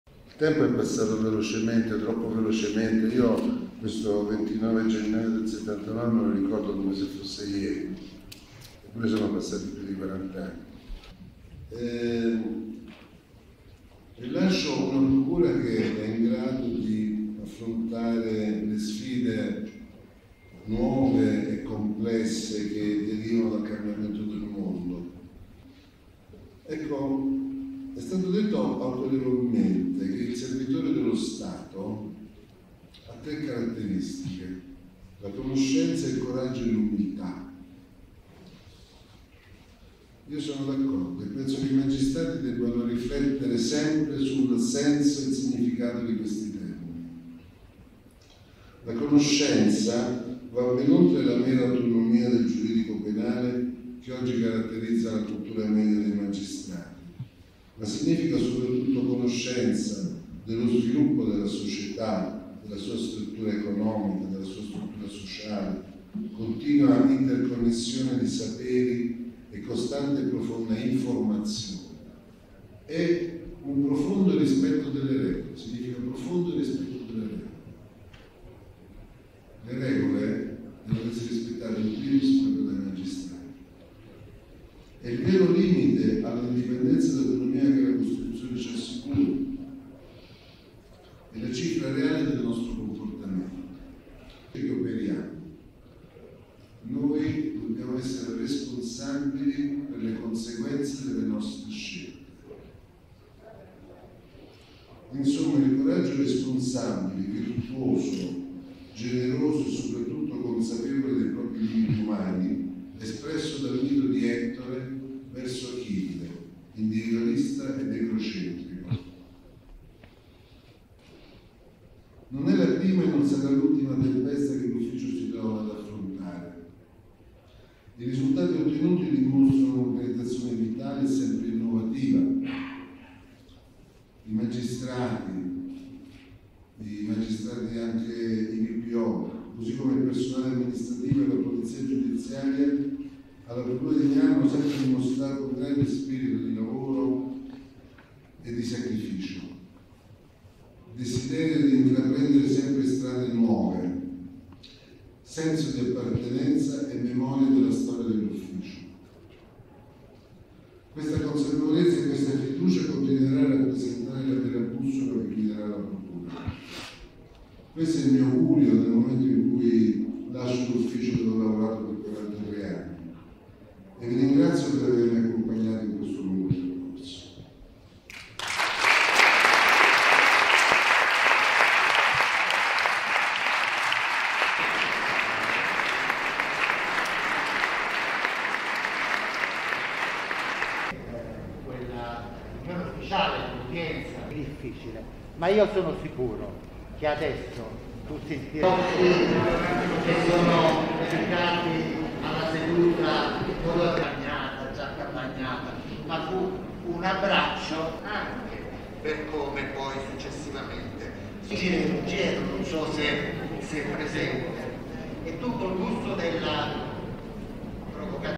Caso pm, Greco: "Le toghe devono rispettare regole"
"Non è la prima e non sarà l'ultima tempesta per la Procura"

 (Milano). Le regole devono essere rispettate in primis dai magistrati". Lo ha detto il procuratore di Milano Francesco Greco, che andrà in pensione sabato, nel suo discorso commosso di saluto nell'aula magna del Palazzo di Giustizia di Milano, facendo anche riferimenti espliciti e impliciti alla 'bufera' che si è abbattuta sulla Procura milanese e allo scontro col pm Paolo Storari, ovviamente non presente al commiato, come anche alcuni altri sostituti procuratori. "Non è la prima e non sarà l'ultima tempesta che l'Ufficio si troverà ad affrontare", ha spiegato ancora Greco. (Francesco Maviglia/alanews)

velocemente troppo velocemente in grado di affrontare le sfide nuove e complesse informazioni Ma io sono sicuro che adesso tutti insieme Un abbraccio e poi successivamente